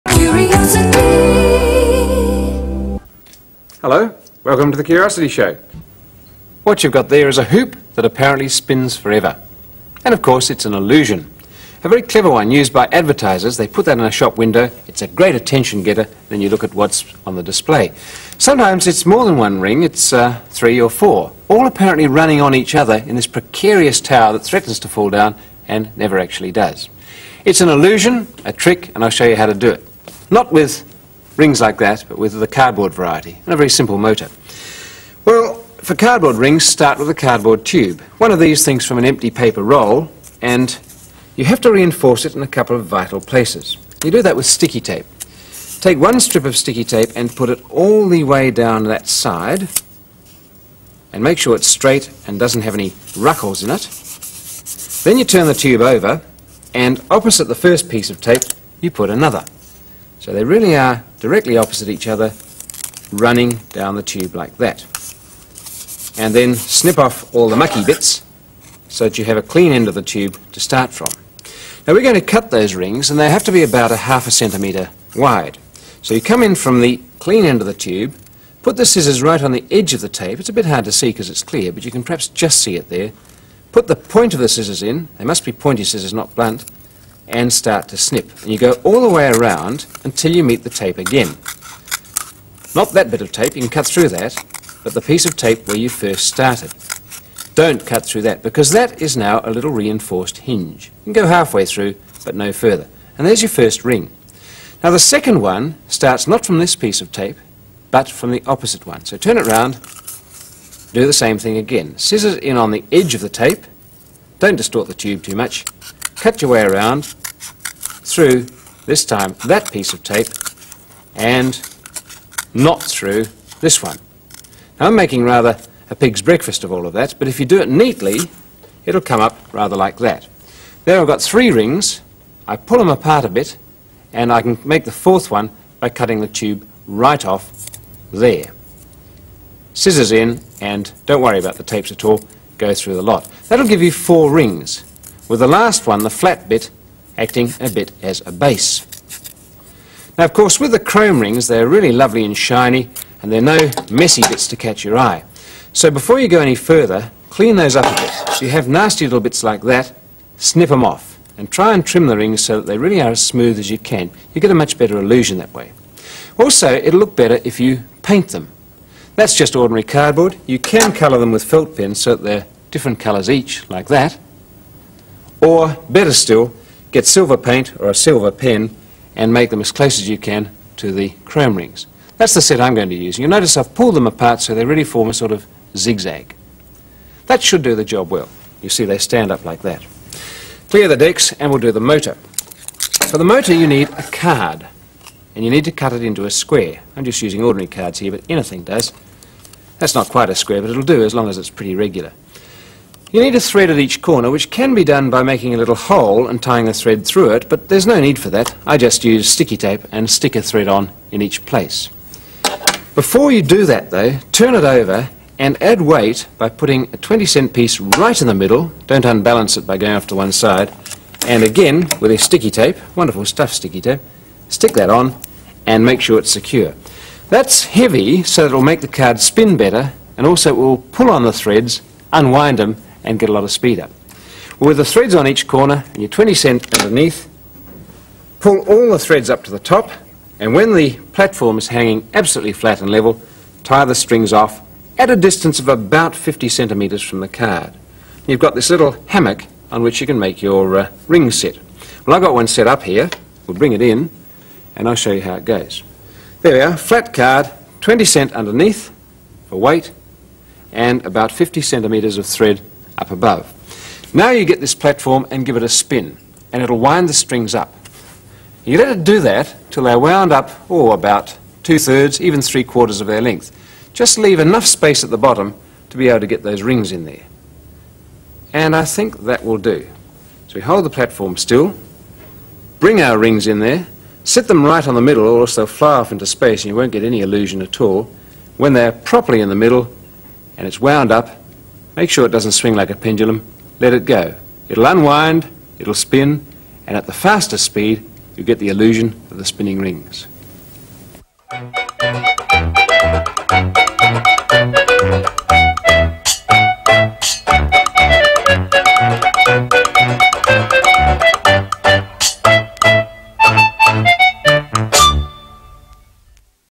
Spinning Rings Illusion To Make
A tower of rings spinning on each other seems impossible but, as Rob shows, it is a terrific illusion and one that you can easily make out of a few simple things around the home.